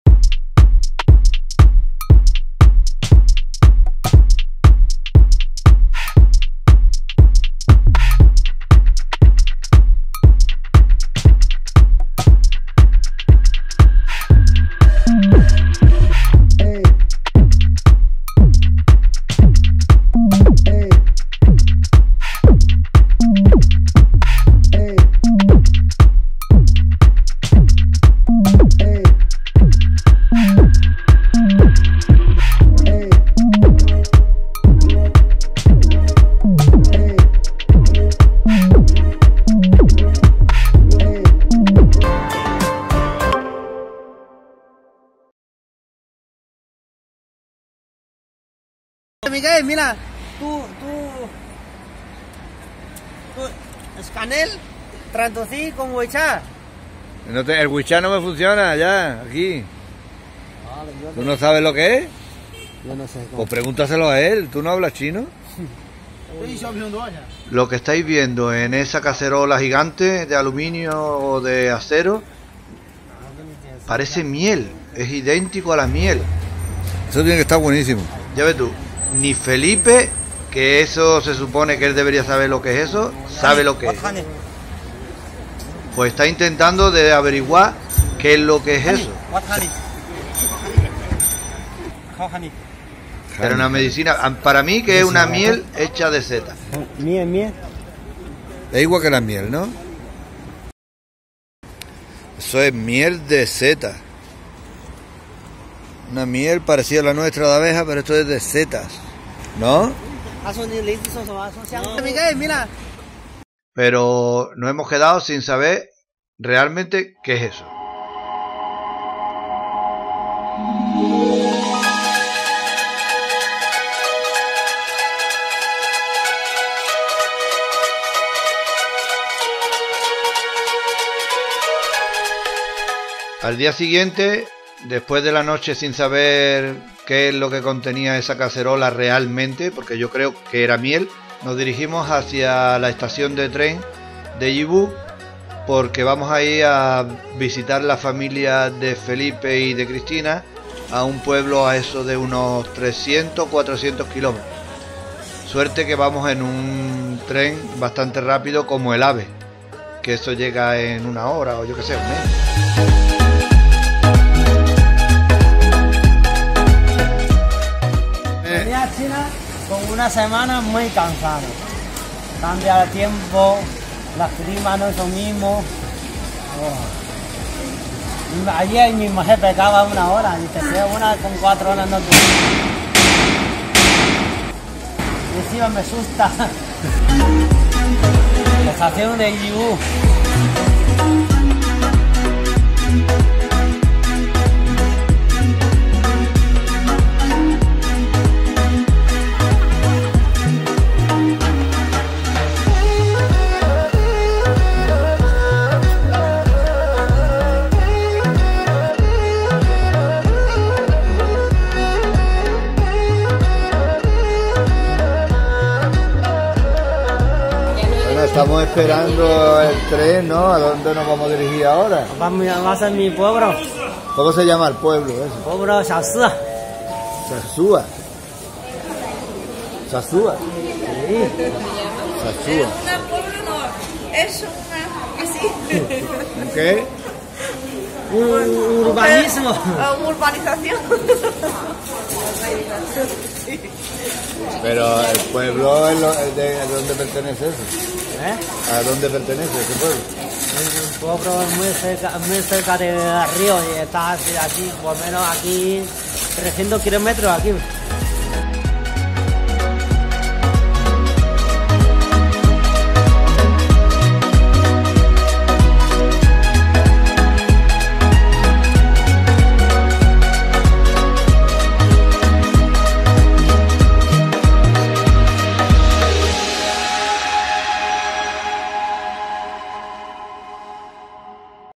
Nuestro viaje a China antes de la pandemia.
Viajar a china es un destino para los viajeros donde la diversidad y la cultura es una situación de asombro, hará de tu viaje una experiencia que te ayudara a comprender el mundo mucho mejor.
Quiero enseñaros una hoya llena de algo parecido a la miel y dentro lleno de setas de todas las clases, queríamos saber si era o no era miel, pero nos quedemos con las ganas, tu sabes que es?